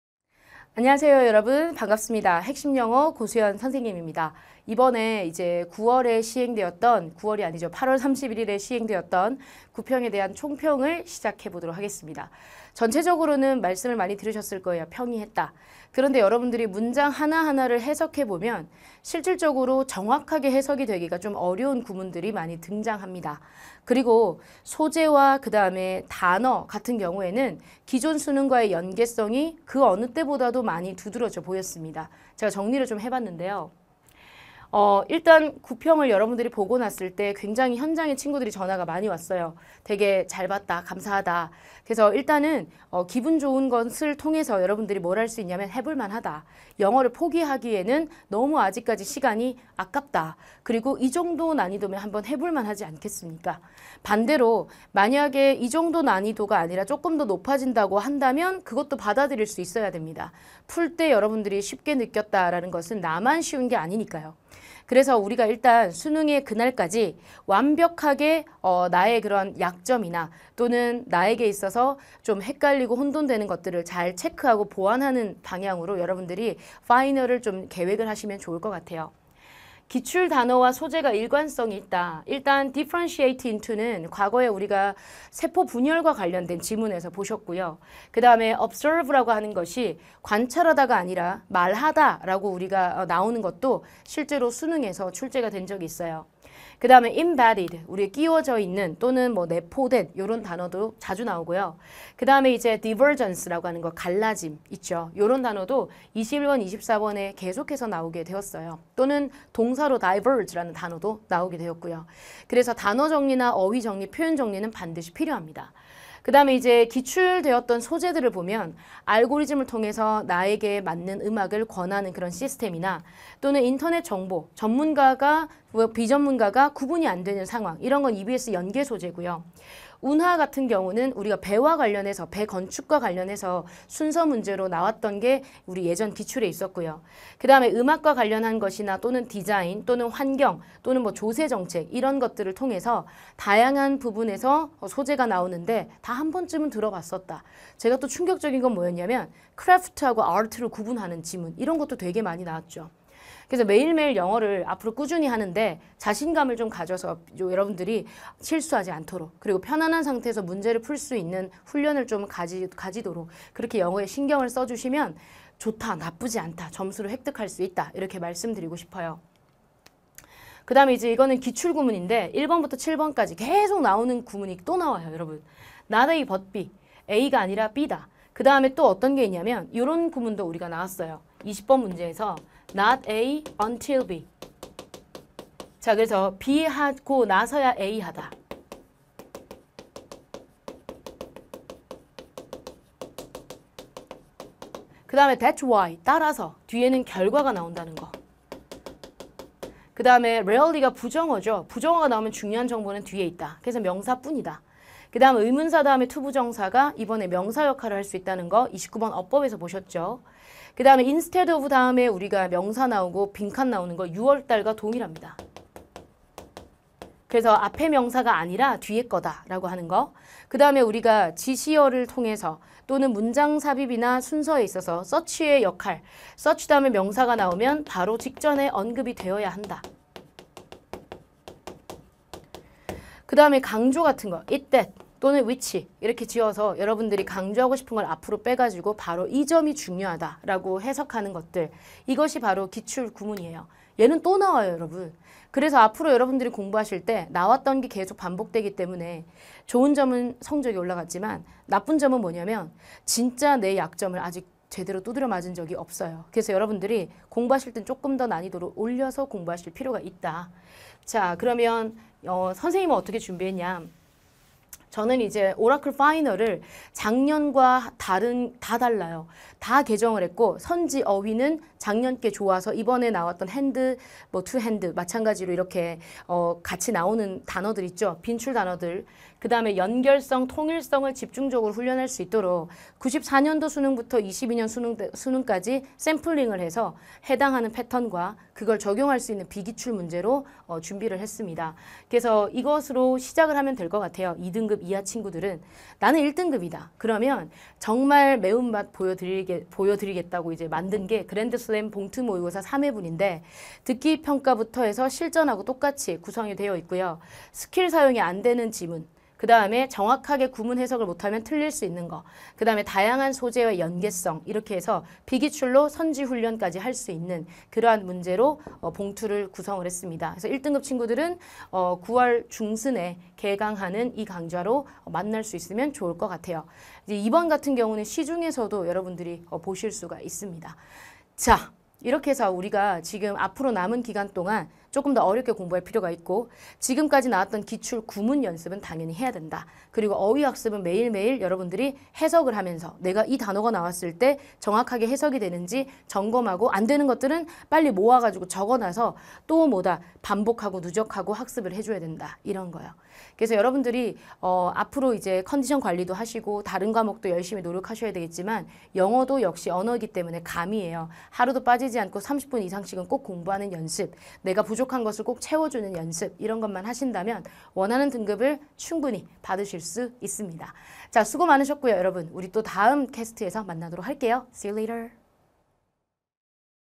[메가스터디] 영어 고수현쌤 - GO수현 선생님의 9월 모의평가 총평
★ GO수현 선생님의 9월 모의평가 영어 총평 ★

# 9월 모평 총평
 1. 해 볼만 하다! 그래도 더 해야 한다!!
 2. 기출 단어와 소재가 일관성이 있다.
 3. 기출에서 이미 나온 소재가 활용되었다.
 4. 매일 '꾸준히'하면서 '자신감'을 가질 것

# 9평 이후의 학습은?
1. 오라클(Oracle) 파이널
 - 선지 어휘, 연결성, 통일성 훈련
 - 비기출 문제 대거 수록 -)) 적용 연습!

2. Grand Slam(그랜드슬램) 봉투 모의고사 3회
 - 스킬 사용이 안되는 경우, 정확한 구문, 소재의 다양성 훈련
 - 선지 훈련(명확한 근거가 없으면 틀리는 문항)
 - 실전과 동일한 시간으로 훈련

여러분 수고 많으셨습니다!
앞으로 원하는 등급을 향해 나아갑시다!

- 영어 고수현 선생님을 더 알고 싶다면?